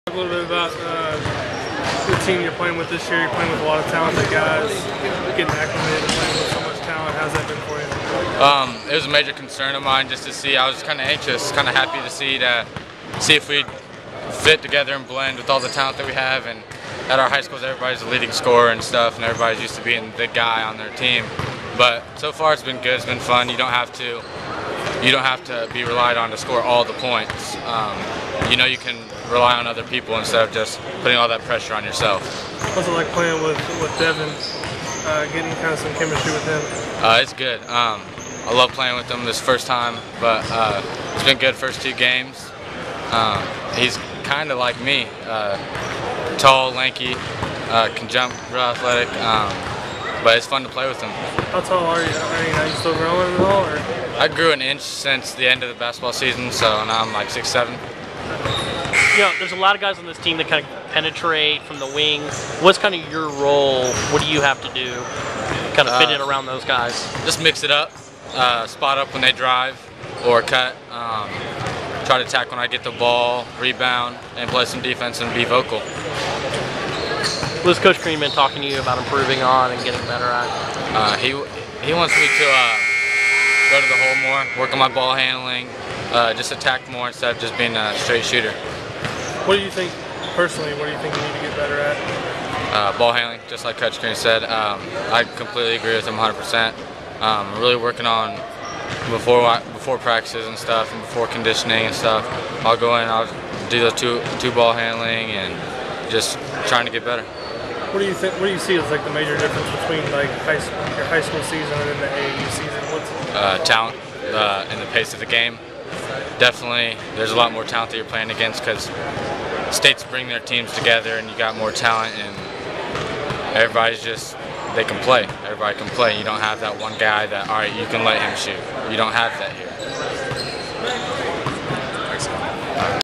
Collin Hartman after pool play of the IndyBall.com Shootout
Indiana commit Collin Hartman after the 16U Eric Gordon All-Stars won the second pool game easily.